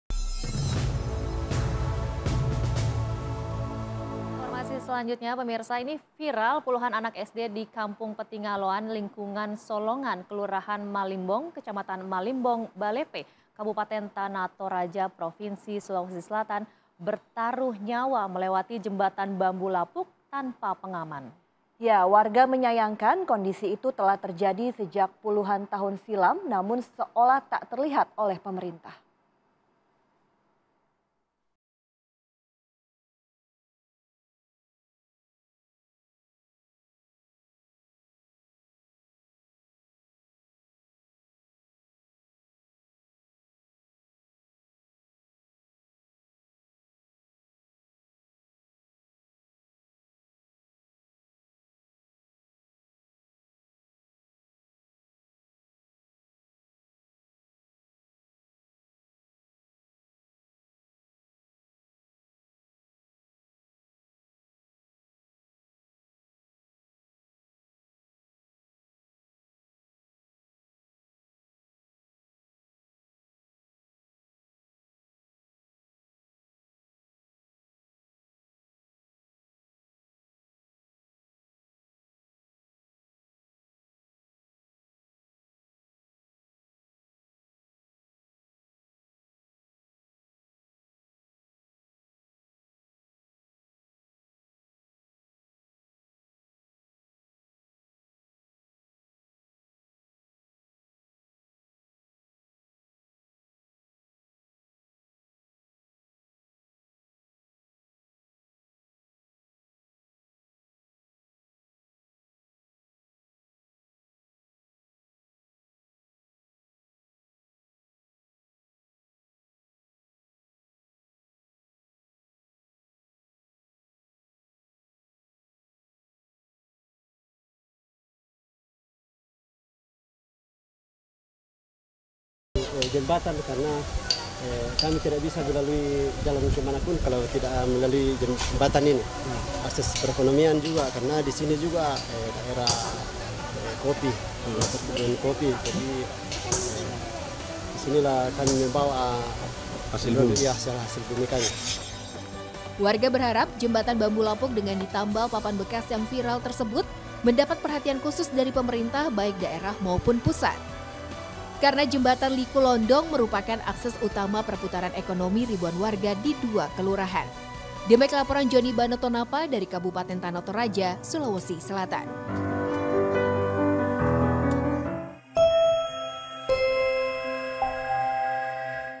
ANAK SD MENANTANG MAUT LINTASI JEMBATAN BAMBU LAPUK
ANTV NEWS+ -

Karena tidak adanya akses menuju ke kesekolahanya beberapa anak SD ini nekat melalui jembatan yang terbuat dari bambu.